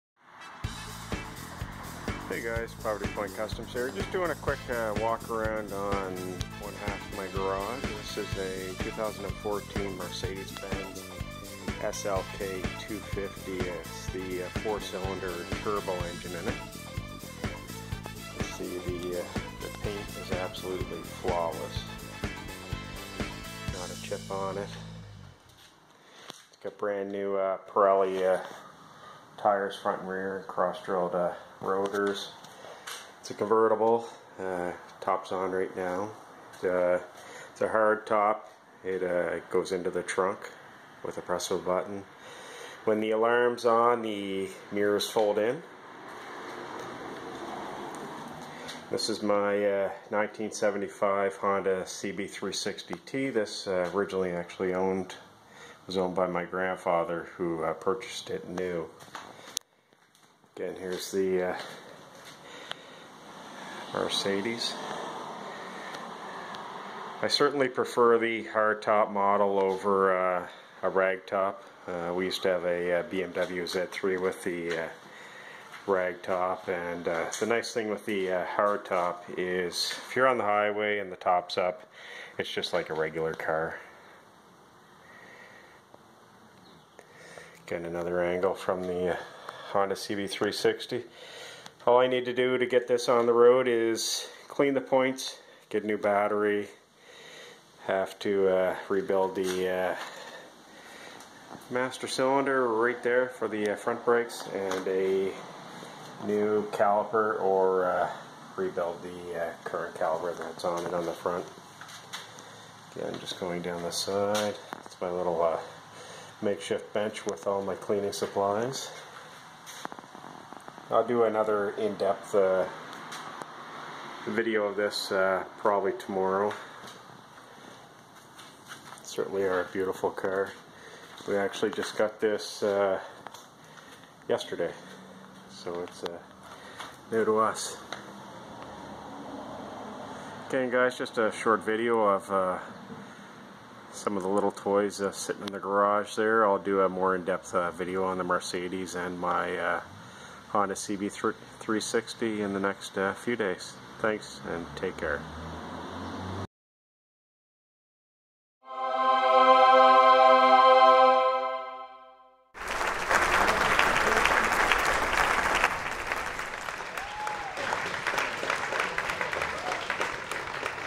2014 Mercedes-Benz SLK 250 and a Honda cb360t
This is a short video of our 2014 Mercedes-Benz SLK 250 and our 1975 Honda cb360t.